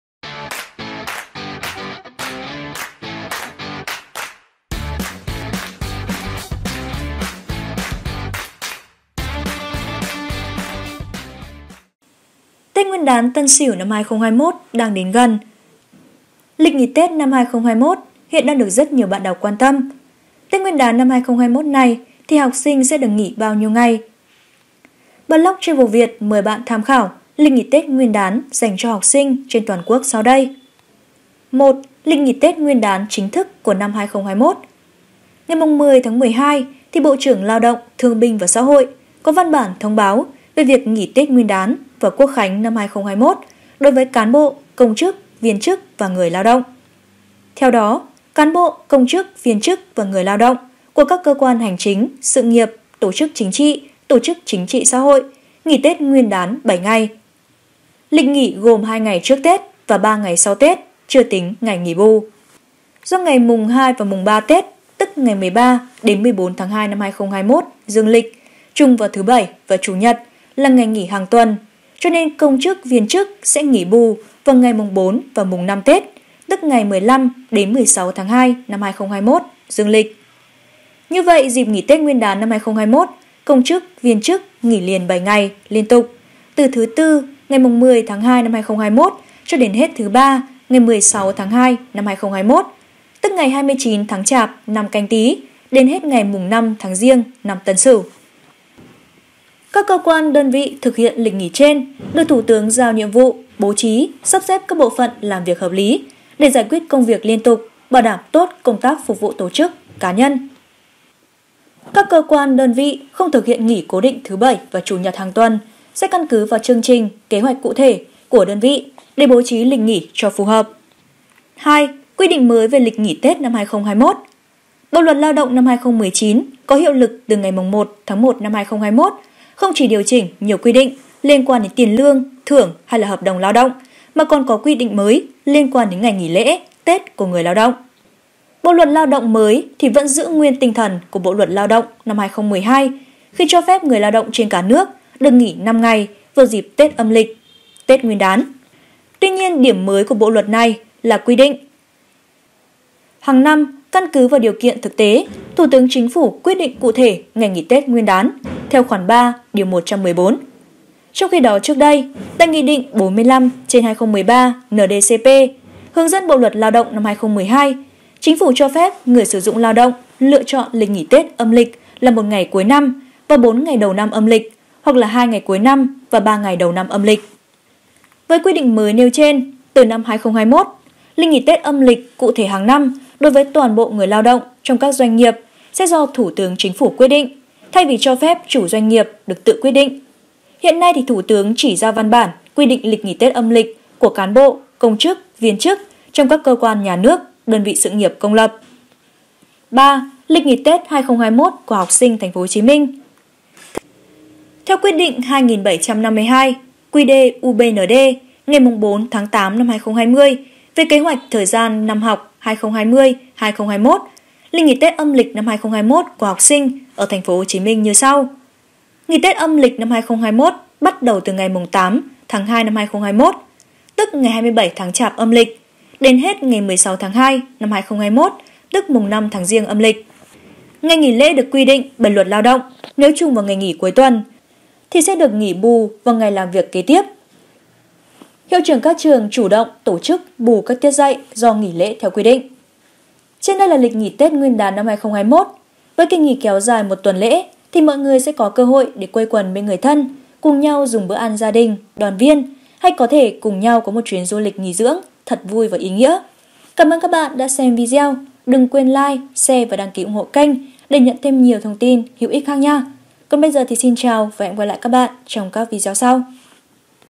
Lịch nghỉ tết chính thức Tân Sửu 2021
Tết Nguyên Đán Tân Sửu 2021 đang đến gần. Lịch nghỉ Tết 2021 hiện đang được rất nhiều bạn đọc quan. Tết nguyên đán 2021 này học sinh sẽ được nghỉ bao nhiêu ngày? HoaTieu.vn mời bạn tham khảo lịch nghỉ Tết nguyên đán dành cho học sinh trên toàn quốc sau đây.

1. Lịch nghỉ Tết Nguyên đán chính thức của năm 2021
Ngày 10-12, Bộ trưởng Lao động - Thương binh và Xã hội có văn bản thông báo về việc nghỉ Tết Nguyên đán và Quốc khánh năm 2021 đối với cán bộ, công chức, viên chức và người lao động.
Theo đó, cán bộ, công chức, viên chức và người lao động của các cơ quan hành chính, sự nghiệp, tổ chức chính trị, tổ chức chính trị-xã hội nghỉ Tết Nguyên đán bảy ngày. Lịch nghỉ gồm hai ngày trước Tết và ba ngày sau Tết (chưa tính ngày nghỉ bù).
Do ngày Mùng 2 và Mùng 3 Tết (tức ngày 13 đến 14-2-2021 Dương lịch) trùng vào thứ bảy và chủ nhật là ngày nghỉ hằng tuần, công chức, viên chức sẽ nghỉ bù vào ngày Mùng 4 và Mùng 5 tết (tức ngày 15 đến-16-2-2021 Dương lịch).
Như vậy, dịp nghỉ Tết Nguyên đán năm 2021, công chức, viên chức nghỉ liền bảy ngày liên tục, từ thứ tư, ngày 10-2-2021 đến hết thứ ba ngày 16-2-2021 (tức ngày 29 tháng Chạp năm Canh Tý đến ngày Mùng 5 tháng Giêng năm Tân Sửu).
Các cơ quan, đơn vị thực hiện lịch nghỉ trên được Thủ tướng giao nhiệm vụ bố trí, sắp xếp các bộ phận làm việc hợp lý để giải quyết công việc liên tục, bảo đảm tốt công tác phục vụ tổ chức, cá nhân.
Các cơ quan, đơn vị không thực hiện nghỉ cố định thứ Bảy và Chủ nhật hằng tuần, sẽ căn cứ vào chương trình, kế hoạch cụ thể của đơn vị để bố trí lịch nghỉ cho phù hợp.

2. Quy định mới về lịch nghỉ Tết 2021
Bộ luật Lao động 2019 có hiệu lực từ ngày 01/01/2021 không chỉ điều chỉnh nhiều quy định liên quan tiền lương, thưởng hay hợp đồng lao động mà còn có quy định mới liên quan đến ngày nghỉ lễ, tết của người lao động.
Bộ luật Lao động mới vẫn giữ nguyên tinh thần của Bộ luật Lao động 2012 khi cho phép người lao động trên cả nước được nghỉ 05 ngày vào dịp Tết Âm lịch (Tết Nguyên đán).
Tuy nhiên, điểm mới của Bộ luật này là quy định: “Hàng năm, căn cứ vào điều kiện thực tế, Thủ tướng Chính phủ quyết định cụ thể ngày nghỉ Tết Nguyên đán” (theo khoản 3 Điều 114).
Trong khi đó trước đây, tại Nghị định 45/2013/NĐ-CP hướng dẫn Bộ luật Lao động 2012, Chính phủ cho phép người sử dụng lao động lựa chọn lịch nghỉ tết Âm lịch là 01 ngày cuối năm và 04 ngày đầu năm âm lịch hoặc 02 ngày cuối năm và 03 ngày đầu năm âm lịch.
Với quy định mới nêu trên, từ năm 2021, lịch nghỉ tết Âm lịch cụ thể hàng năm đối với toàn bộ người lao động trong các doanh nghiệp sẽ do Thủ tướng Chính phủ quyết định, thay vì cho phép chủ doanh nghiệp được tự quyết định (hiện nay, Thủ tướng chỉ ra văn bản quy định lịch nghỉ tết Âm lịch của cán bộ, công chức, viên chức trong các cơ quan Nhà nước, đơn vị sự nghiệp công lập).

3. Lịch nghỉ Tết 2021 của học sinh TP.HCM
Theo Quyết định 2752/QĐ-UBND ngày 04/8/2020 về Kế hoạch thời gian năm học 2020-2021, lịch nghỉ tết Âm lịch 2021 của học sinh ở TP.HCM như sau:
Nghỉ Tết Âm lịch 2021 bắt đầu từ ngày 08/02/2021 (27 tháng Chạp Âm lịch) đến hết ngày 16/02/2021 (Mùng 05 tháng Giêng Âm lịch).
Ngày nghỉ lễ được quy định bởi Luật Lao động nếu trùng vào ngày nghỉ cuối tuần thì được nghỉ bù vào ngày làm việc kế tiếp. Hiệu trưởng các trường chủ động tổ chức bù các tiết dạy do nghỉ lễ theo quy định.
Trên đây là lịch nghỉ tết nguyên đán 2021. Với kỳ nghỉ kéo dài 1 tuần lễ thì mọi người sẽ co cơ hội quây quần bên người thân để cùng nhau dùng bữa ăn gia đình đoàn viên hay có thể cùng nhau có 1 chuyến du lịch nghỉ dưỡng thật vui và ý nghĩa. Xin chào và hẹn gặp lại trong các vidoe sau.